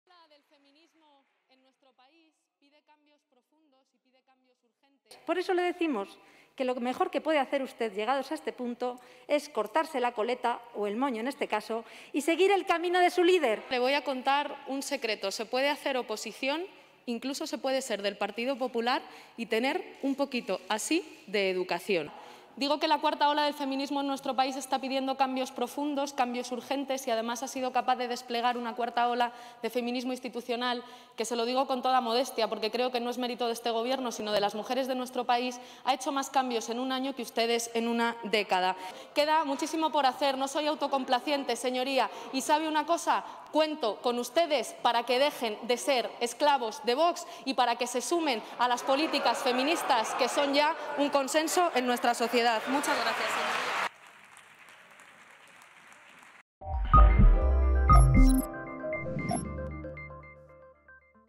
Montero, a Patricia Rodríguez: "Se puede hacer oposición y ser del PP y tener un poco de educación"
Tras la 'recomendación' de Patricia Rodríguez (PP) a Irene Montero de cortarse la coleta y seguir el camino de su líder, la ministra ha contestado así: "Se puede hacer oposición y ser del PP y tener un poco de educación".